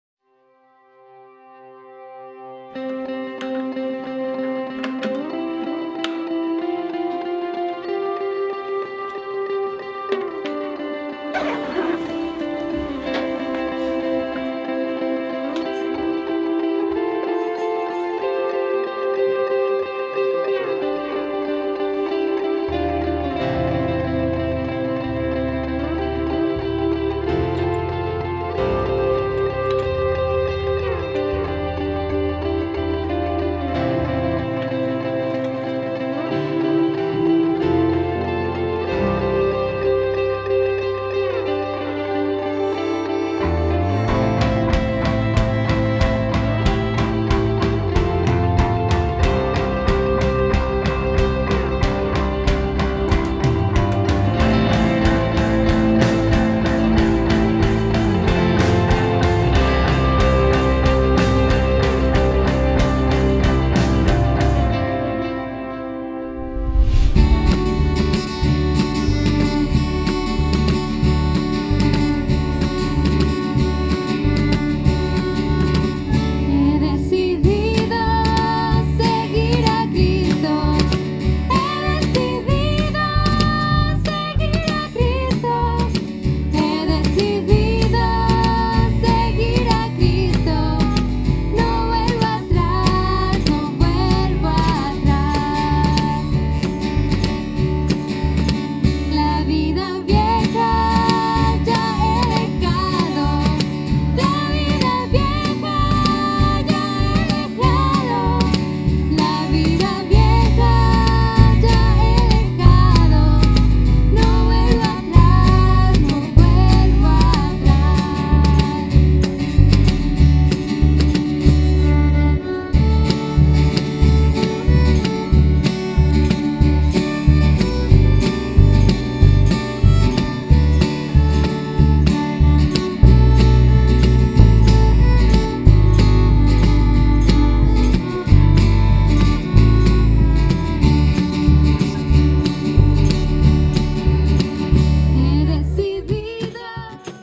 The Echoes Music Team - Rio Grande Bible Ministries
The Echoes Music Team 2020! Every year the Echoes tour North America to encourage the church and praise the Lord through their music. Their goal is to assist in edifying believers and evangelizing the lost.